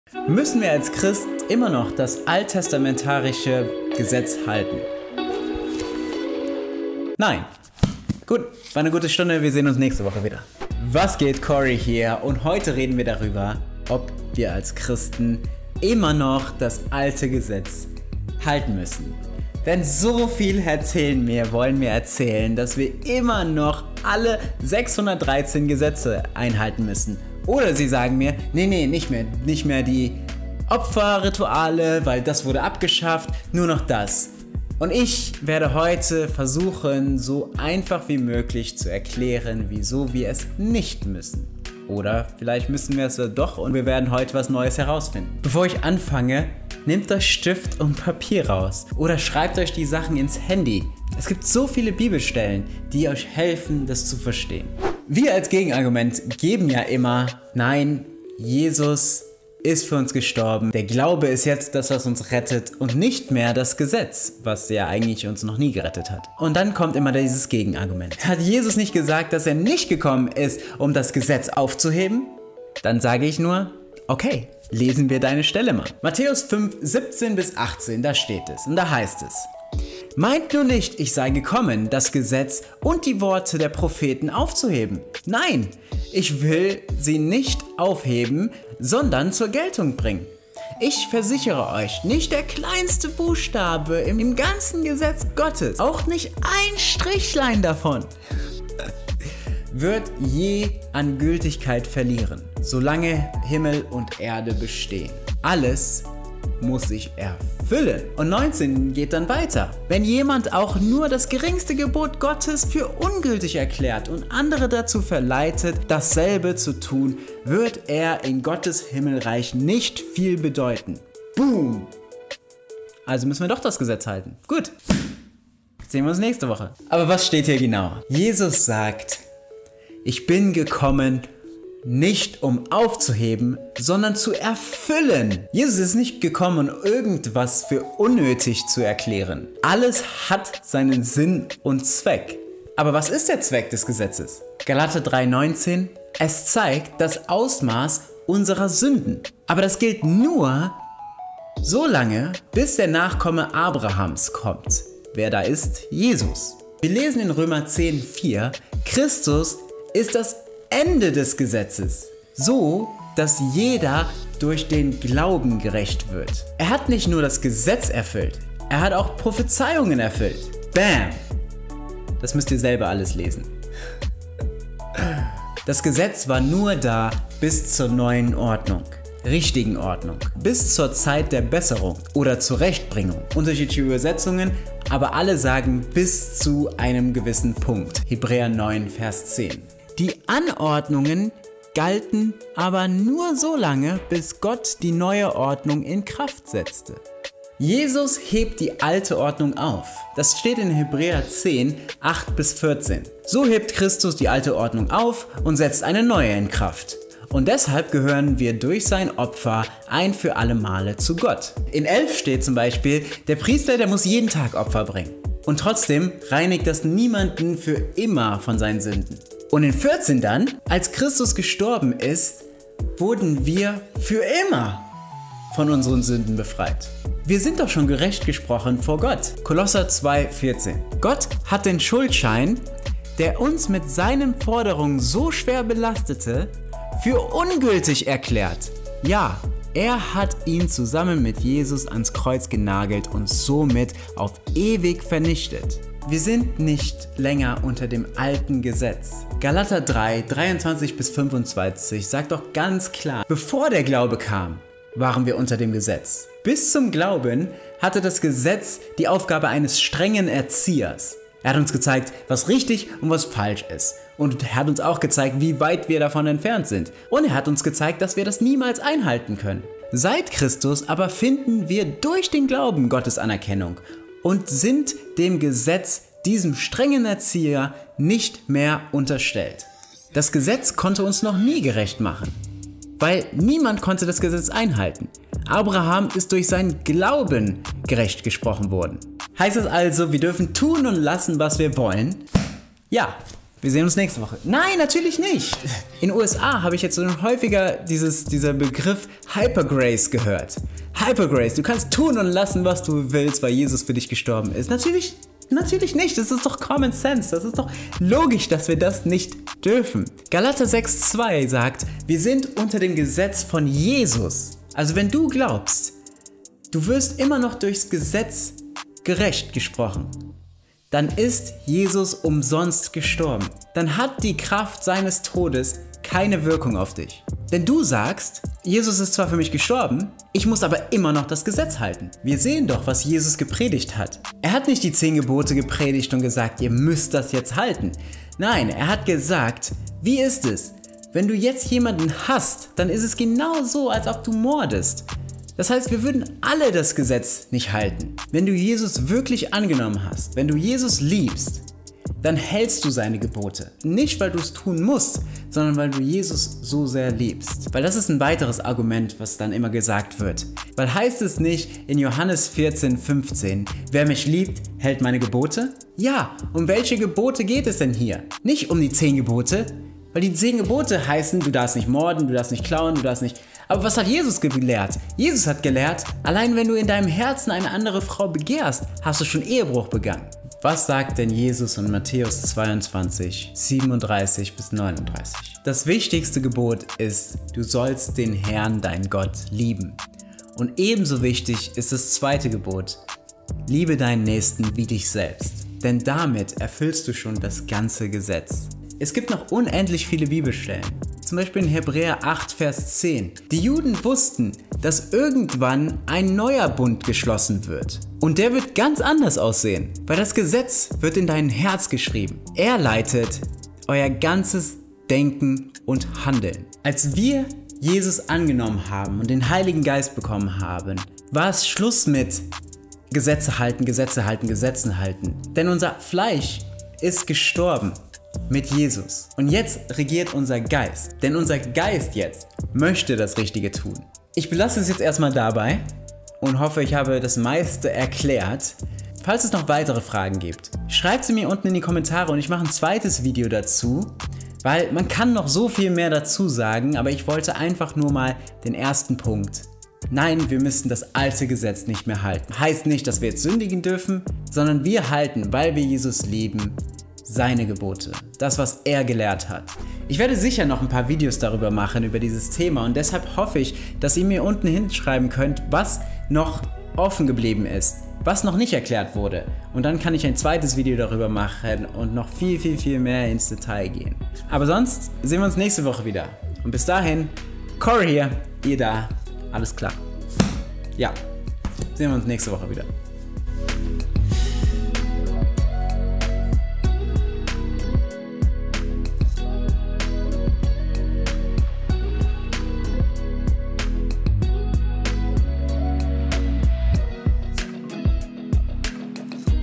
Müssen Christen das Gesetz aus dem alten Testament halten?
Müssen wir die 10 Gebote und / oder die weiteren Gebote aus dem alten Testament halten? Was sagt die Bibel dazu?

Du kannst außerdem das Video liken & teilen. Stell mir gerne Fragen! Ich freue mich, mit dir ins Gespräch zu kommen.